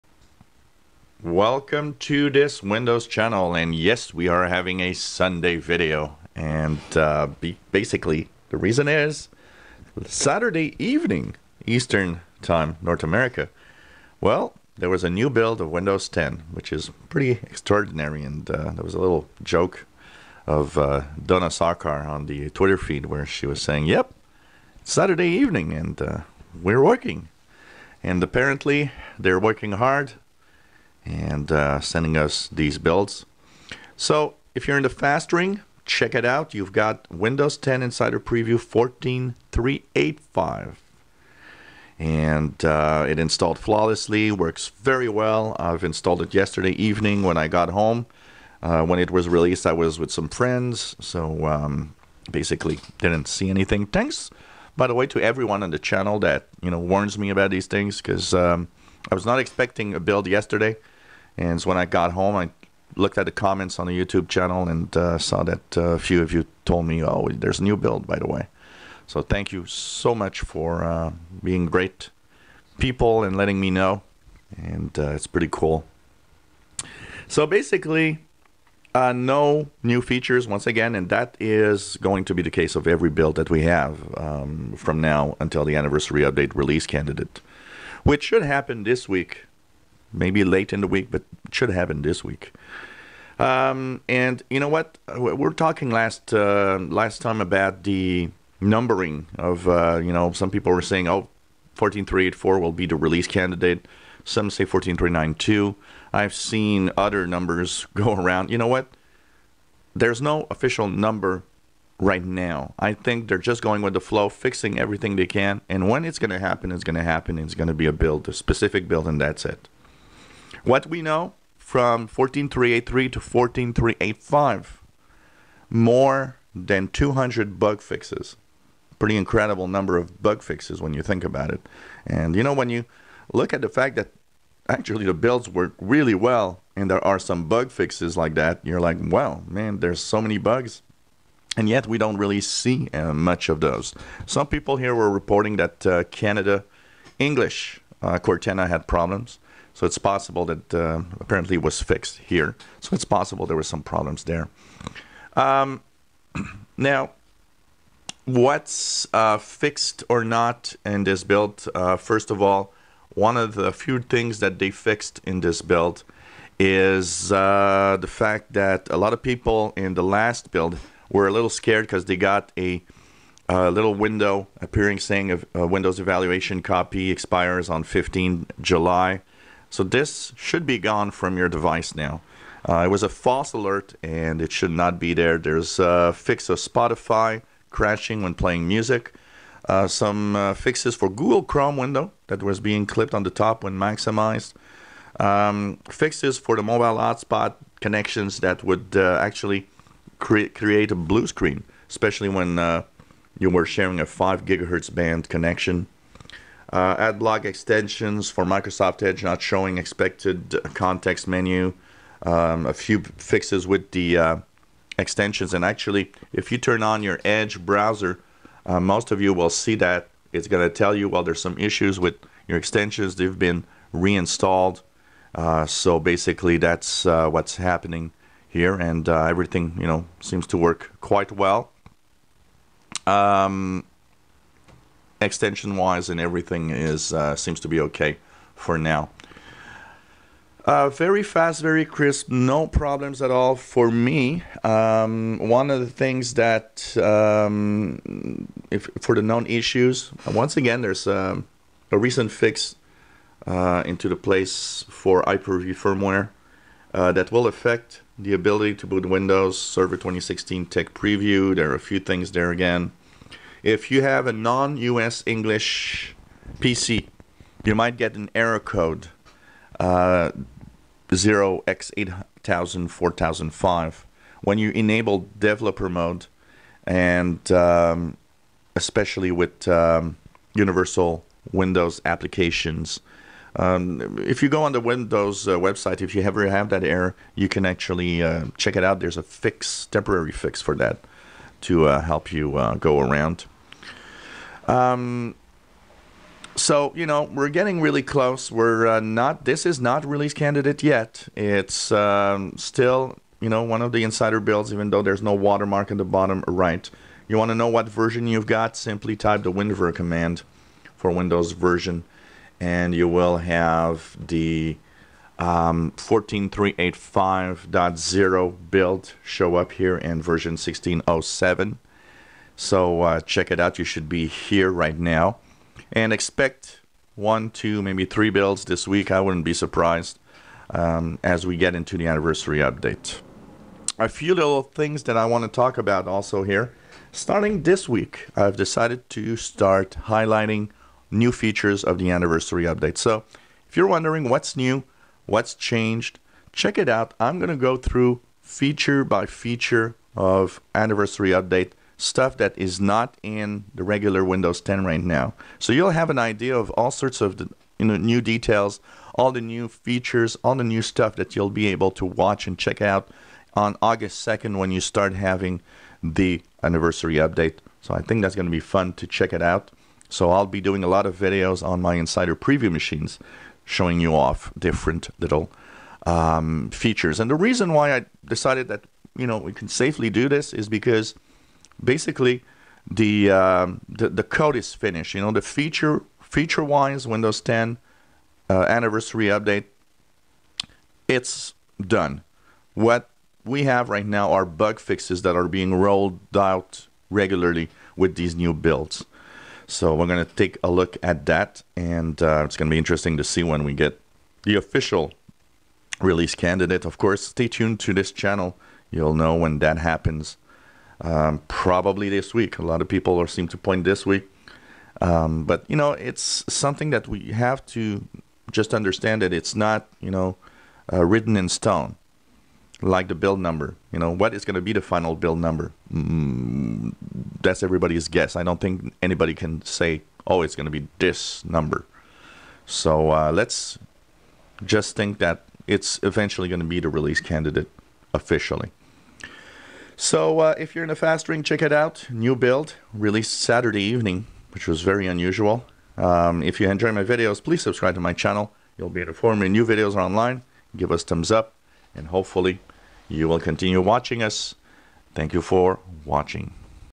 Windows 10 Insider preview Build 14385 Observations and comments July 10th 2016
A new build for insiders is now available for the Fast ring, it was released on Saturday ÀJuly 9th 2016